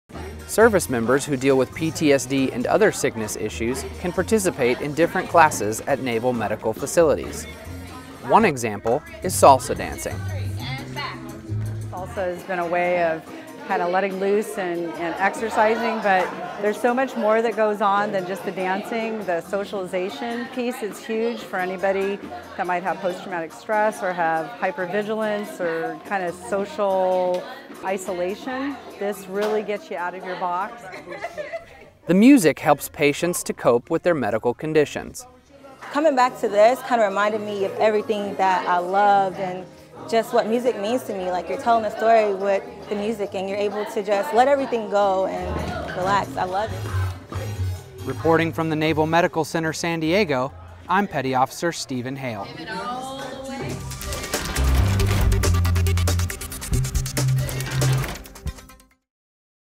Salsa Dancing
Servicemembers who deal with PTSD and other sickness issues participated in a Salsa Dancing classs at Naval Hospital San Diego.